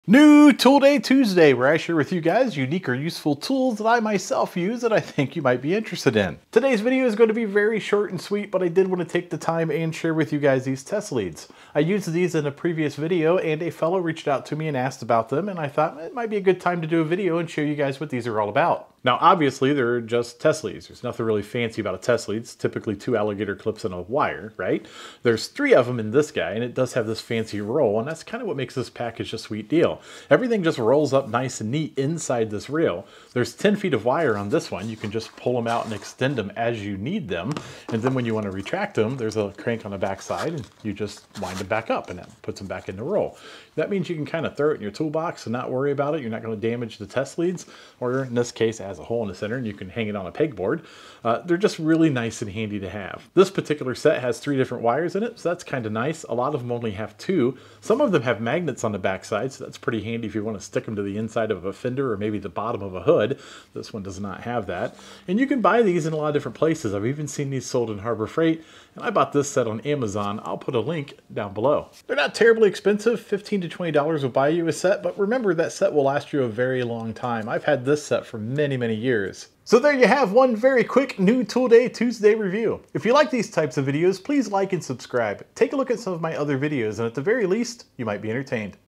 Retractable Test Leads, Alligator to Alligator clips, New Tool Day Tuesday!
- Lang Tools (1129 Retractable Test Lead, black, 3 Leads X 10'

From the description: The Lang Tools Retractable Test Leads - 3 Leads x 10 feet is used for testing electrical circuits in cars, trailers, boats, and more. The retractable test leads pull out and stay at desired length and rewind into its own protective case when not in use. These leads feature 18 gauge copper wire with insulated alligator clips.

! ! ! DISCOUNTS ! ! !